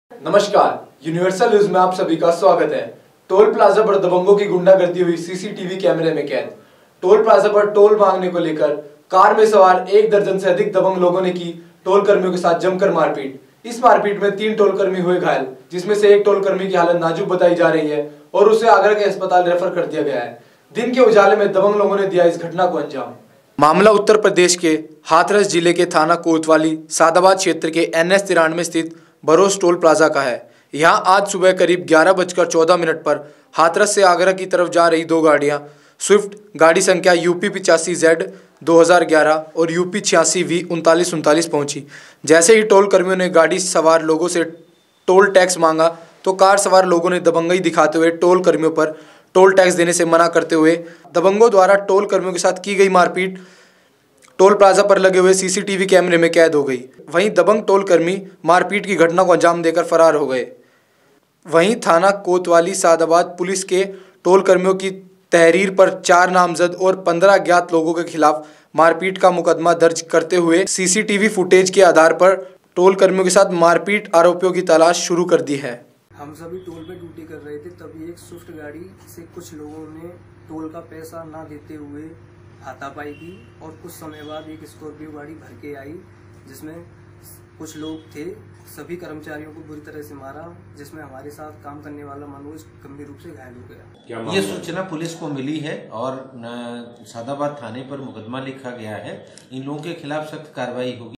टोल प्लाजा पर दबंगो की गुंडई हुई CCTV कैमरे में कैद | Toll Plaza Par Dabango Ki Gundai | Hathras
मामला उत्तर प्रदेश के हाथरस जिले के थाना कोतवाली सादाबाद क्षेत्र के NH-93 स्थित बरोस टोल प्लाजा का है। यहां आज सुबह करीब 11 बजकर 14 मिनट पर हाथरस से आगरा की तरफ जा रही दो गाड़िया। स्विफ़्ट गाड़ी संख्या UP85 Z 2011 और UP86 V 3939 पंहुची। जैसे ही टोल कर्मियों ने गाड़ी सवार लोगो से टोल टैक्स माँगा तो कार सवार लोगो ने दबंगई दिखाते हुये टोल कर्मियों को टोल टैक्स देने से मना करते हुये दबंग टोल कर्मियों के साथ मारपीट करने लगे। दबंगो दवारा टोल कर्मियों के साथ की गयी मारपीट की पूरी वारदात टोल प्लाजा पर लगे हुये CCTV कैमरे में कैद हो गयी। वंही दबंग टोल कर्मियों  मारपीट की घटना को अंजाम देकर फरार हो गये।